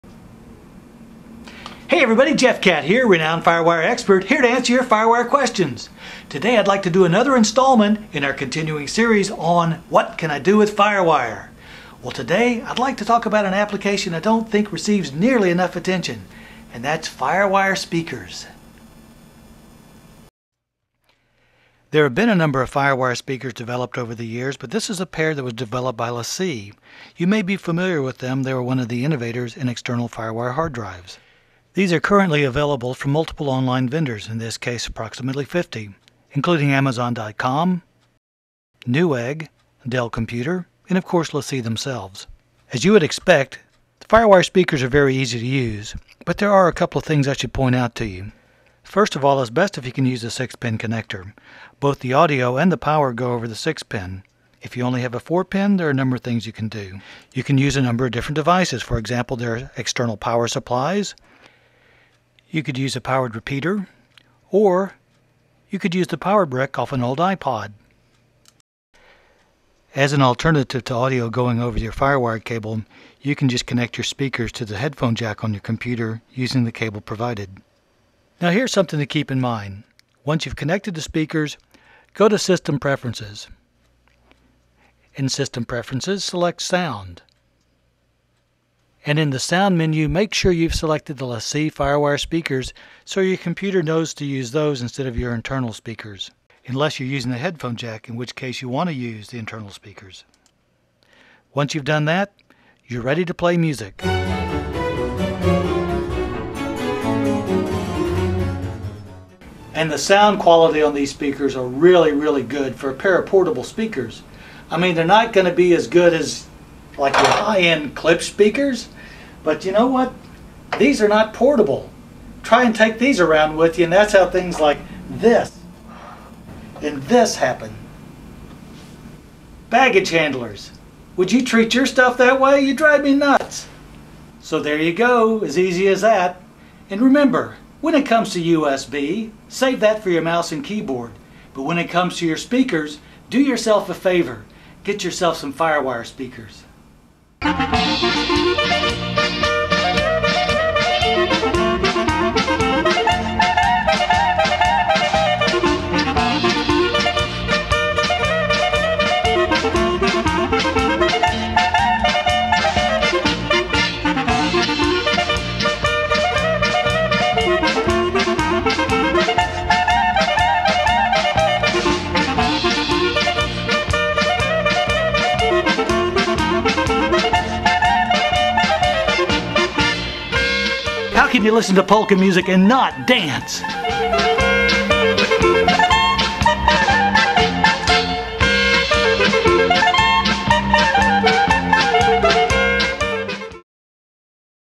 What Can I Do with FireWire 1394? part 2
FireWire Expert gives second answer to question "What can I do with FireWire?"